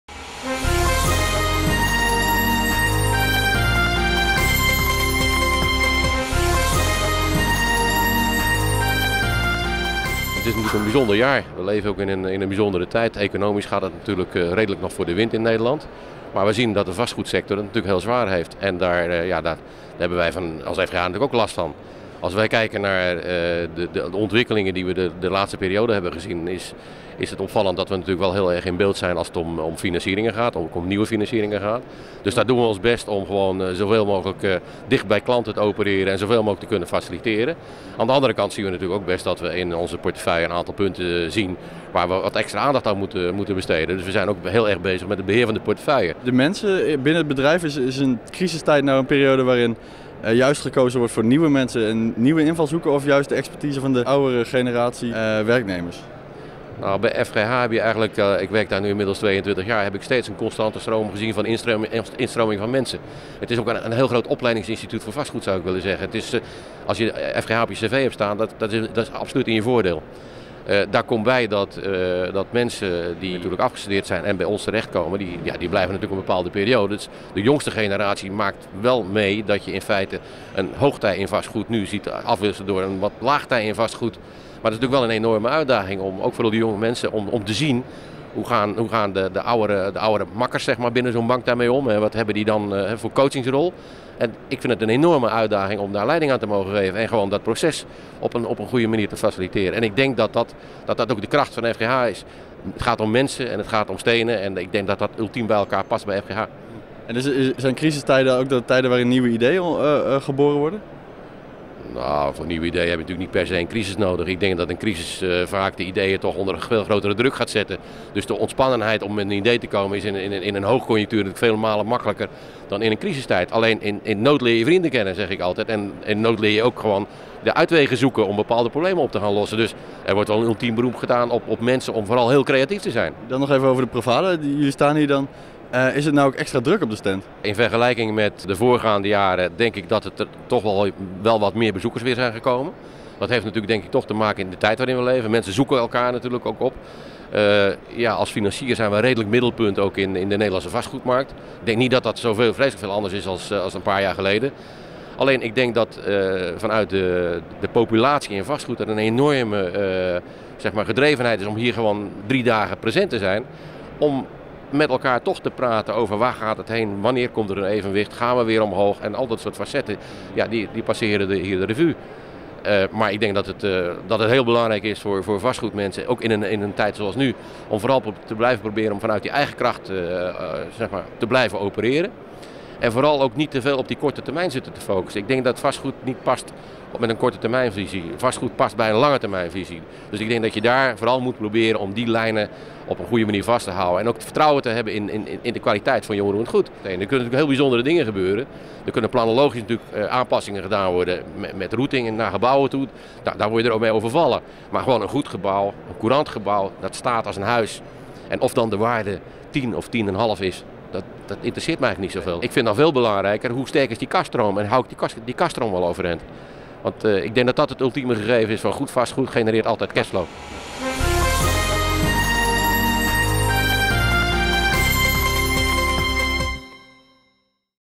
Peter Keur (FGH Bank) PROVADA 2011
Vastgoedmarkt interview met Peter Keur, directievoorzitter FGH Bank.
Het interview is opgenomen tijdens de PROVADA 2011, het ontmoetingspunt voor professionals in commercieel vastgoed.